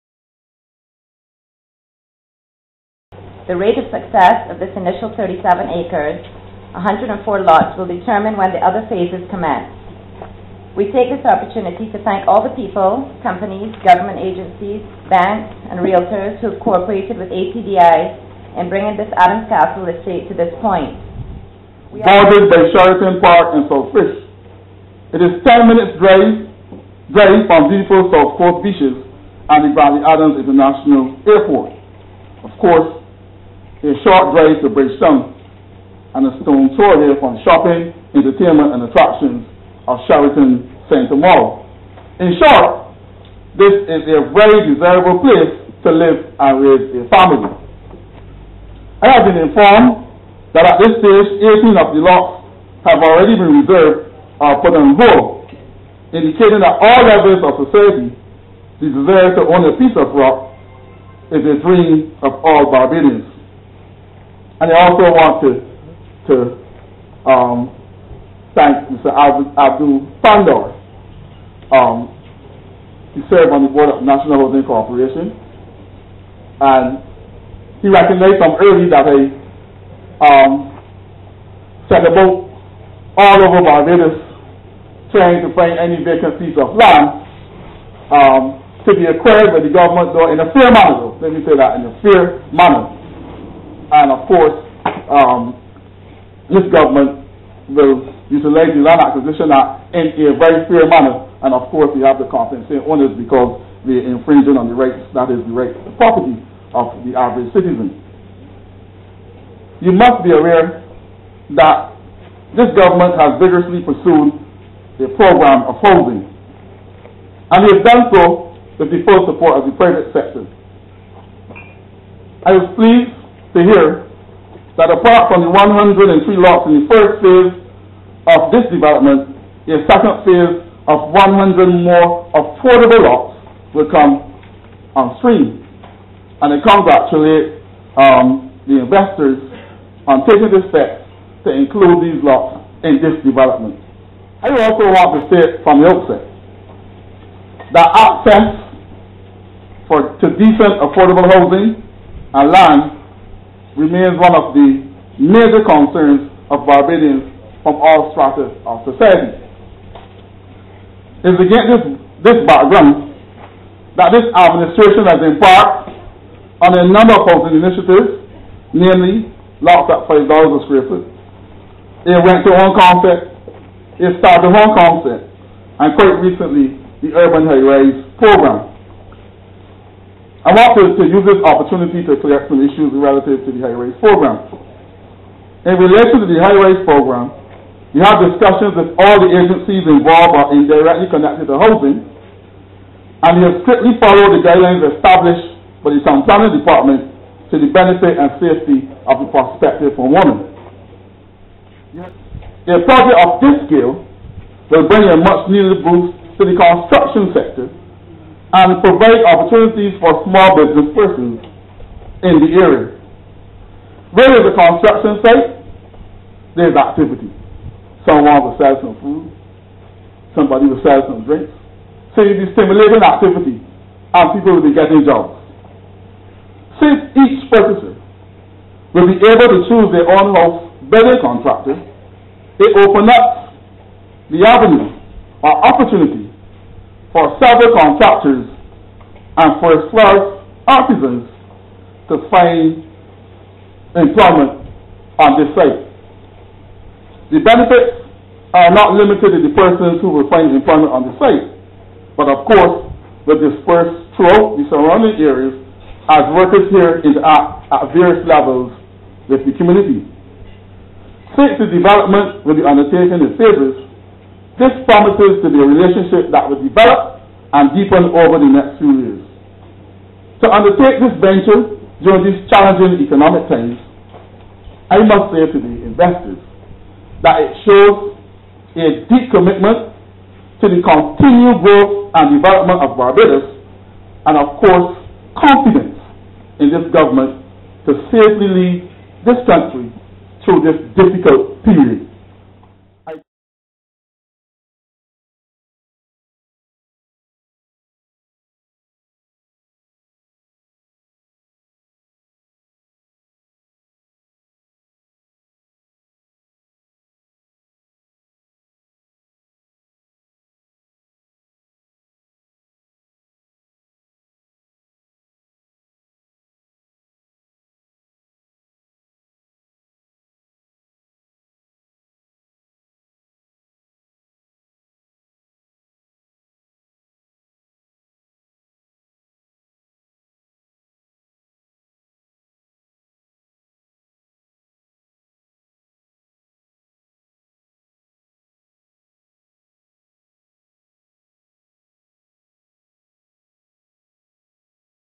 New Barbadian Gated Community - Adams Castle Estate: Very Upscale
Two gates heavily monitored surrounding 104 lots where lots are awaiting sales and development into homes & properties for some of Barbados but it remains to be seen if any Bajan can claim a piece of this part of the Rock?

Most Utilities are slated to be stored under the development's surface, this sophistication probably explains why many Lots average at a common rate of Approximately $375,000 per Unit - please view feature right to end where the Credits detail the High & Low end of areas which do not even have a single brick laid or stick of furniture to be moved in, since there are not yet roofs to shelter!

The quality of dwellings can also raise or lower the value of a given property, with Rayside Construction looking to secure most of the contracts, one can be assured the edifices will be developed from a rather significant budget which may indeed require rather Deep Pockets in order to see reality...

This in fact begs the question as to how many Bajans, in this current and long-outstanding Fiscal Crisis, can truly end up calling any of these properties as Home? With pricing like this, one cannot be blamed if they pondered if this is te South Coast's answer to the West Coast off-duty Hollywood lifestyle?